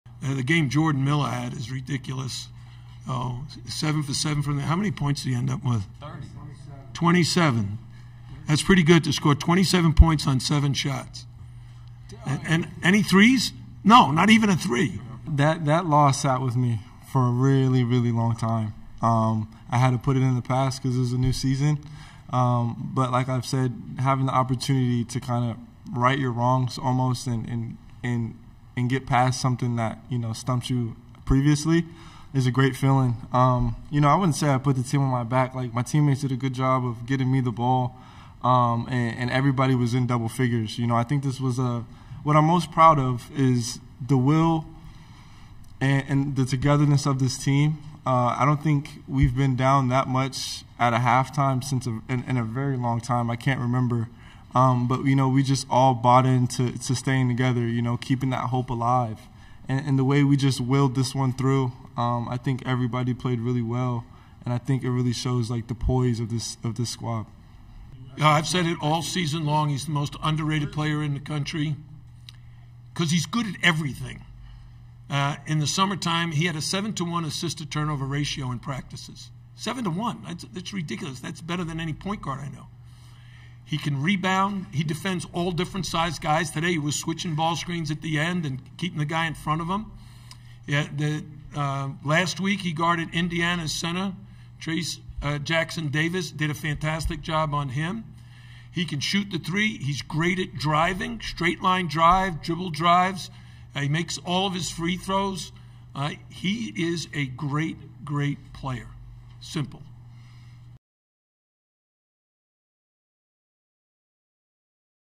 Jordan Miller's Perfect Night For Miami
Jordan Miller was a perfect 7-7 from the field and 13-13 from the free throw line as he led Miami past Texas and onto the Hurricanes' first trip to the Final Four. Coach Jim Larrañaga talked after the game about how fantastic he has been all season long, and Miller spoke about how last year's Elite Eight loss motivated him for tonight.